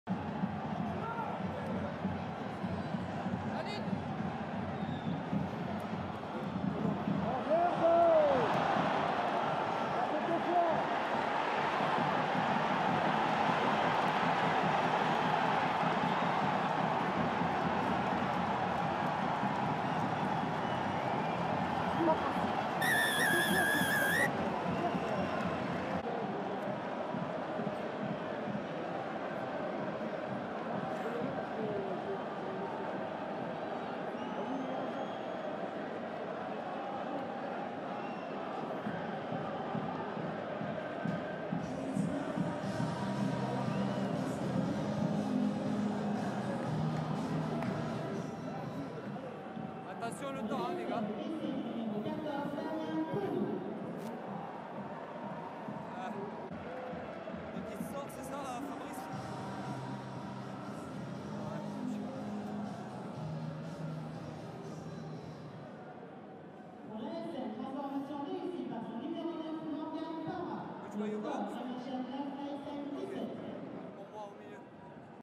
TOP 14 - L'image de la J11
Quand Damian Penaud de Clermont réceptionne un ballon dans ses 22 mètres, passe en revue toute la ligne défensive de Montpellier et traverse le terrain pour aller marquer un magnifique essai !